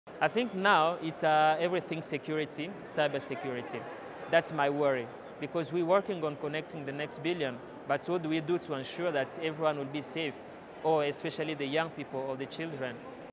Global IGF 2016: Arsene Tungali on Top Internet Issues
Arsene Tungali, CEO, Rudi International, shares his answer to the question, “What are the most difficult Internet challenges, fears or issues that must be addressed to assure the best future for all?” Recorded at the Global Internet Governance Forum 2016 in Guadalajara, Mexico, by the Imagining the Internet Center, an initiative of Elon University, North Carolina, USA.